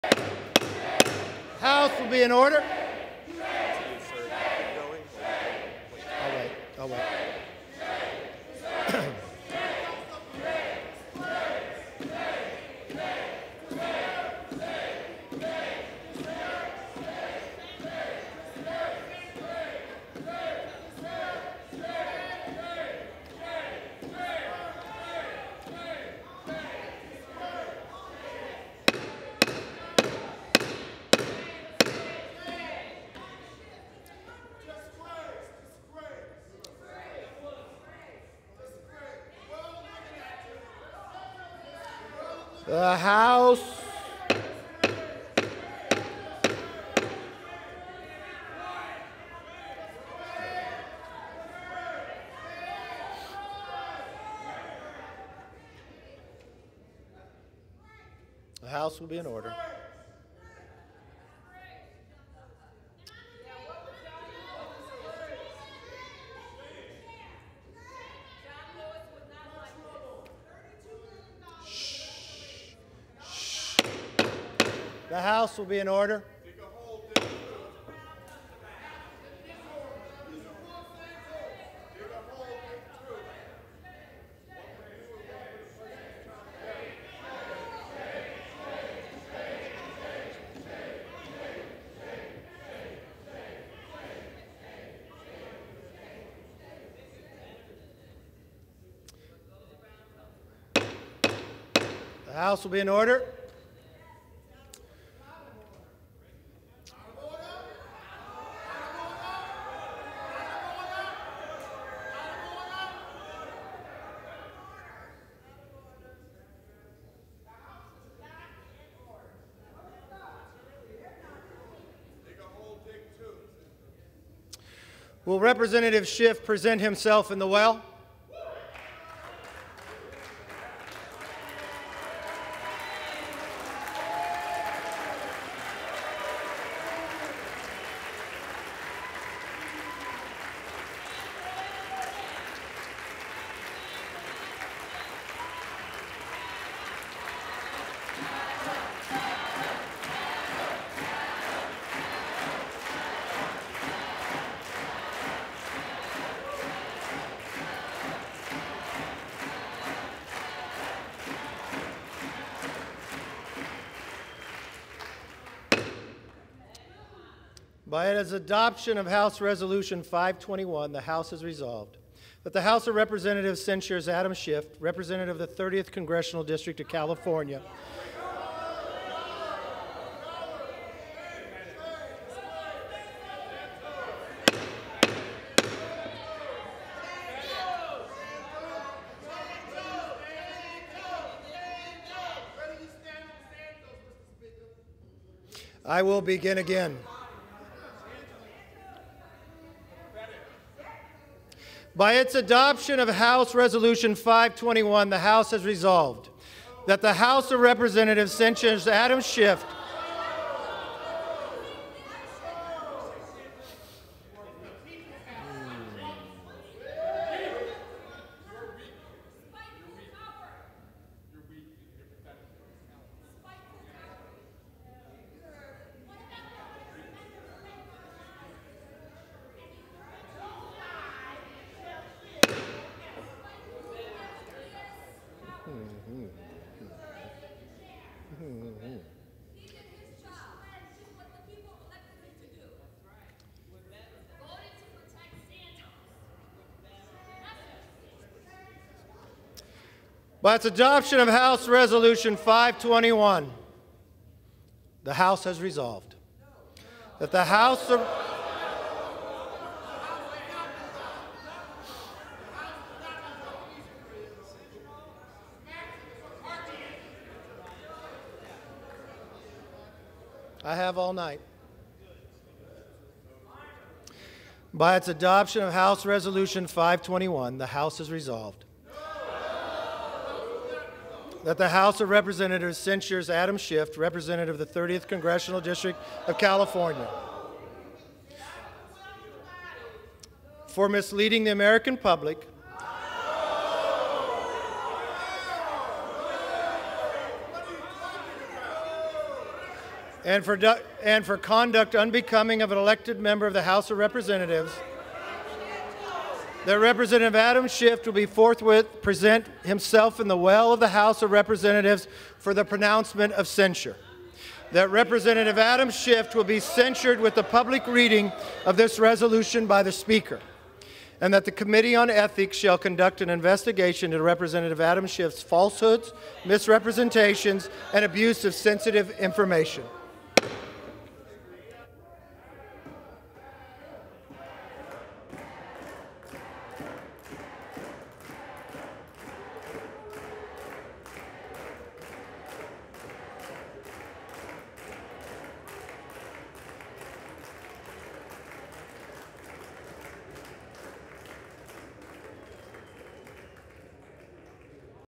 See Democrats repeatedly interrupt McCarthy on House floor
Democrats on the House floor interrupt Speaker Kevin McCarthy after lawmakers voted to censure Rep. Adam Schiff (D-CA), a key lawmaker in Democrats' congressional investigations into former President Donald Trump during his presidency.